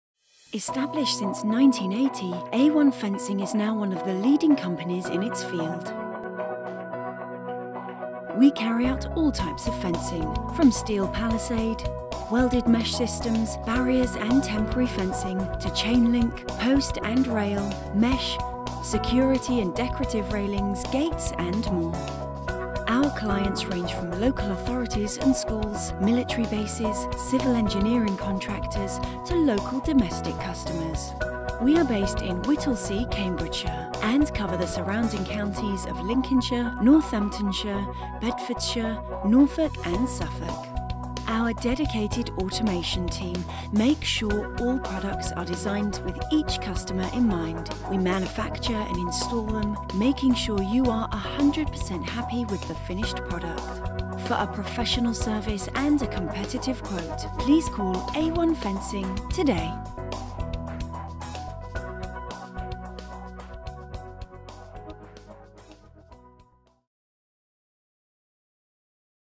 Fencing & Gate Products - A1 Fencing
A1 Fencing carries out all types of fencing for commercial, industrial, security, domestic or equestrian uses. These include: steel palisade, ultra pale palisade, welded mesh systems, barriers and temporary fencing, chain link, post and rail, mesh, close board, panels and trellis, security and decorative railings and gates, and gate automation. Large quantities of fencing and gates are available from stock.